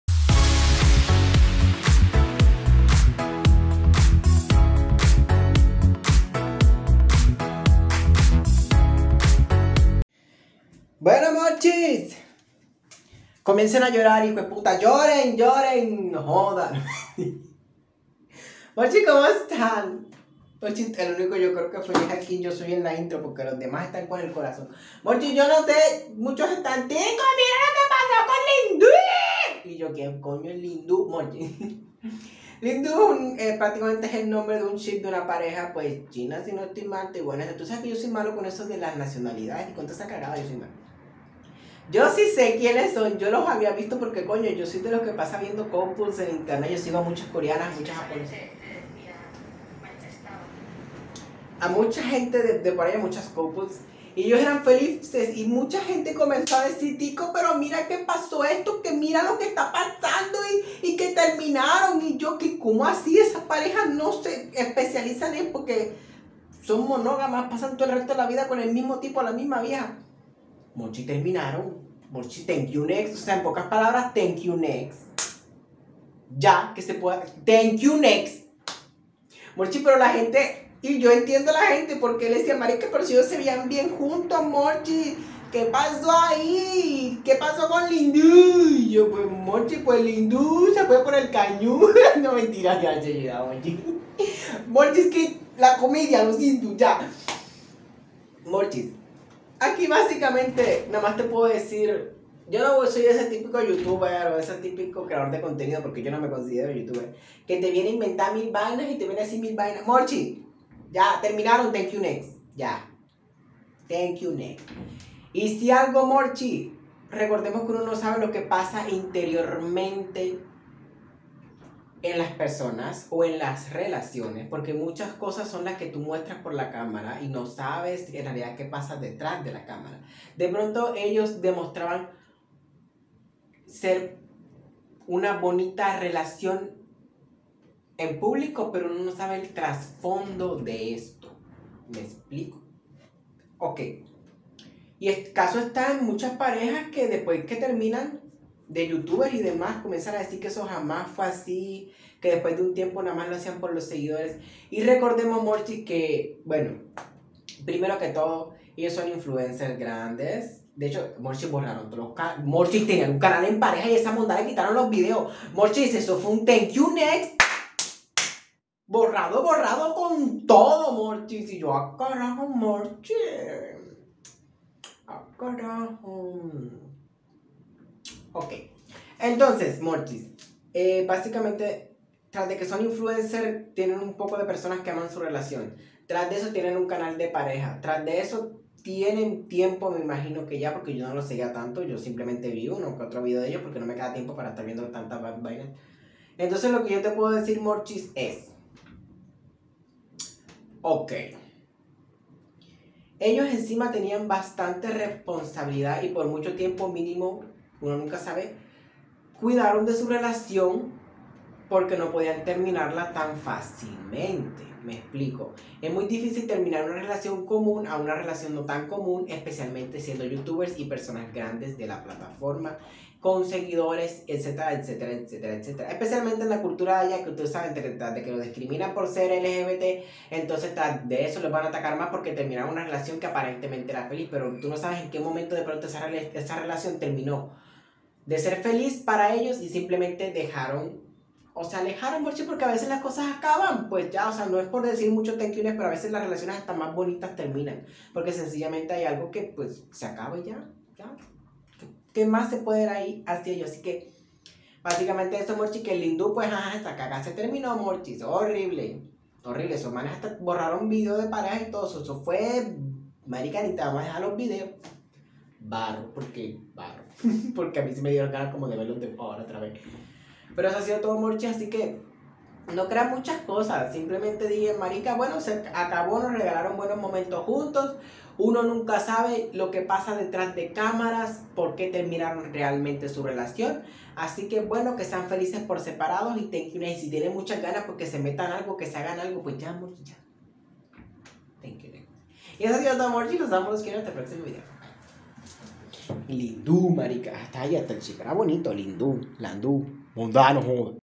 LinDu TERMINA OFICIAL MENTE !💔 | ELIMINAN TODO DE REDES ?💔 | POR QUE ?💔 | TiCo-TK
PUEDES SEGUIRME EN !!! 💌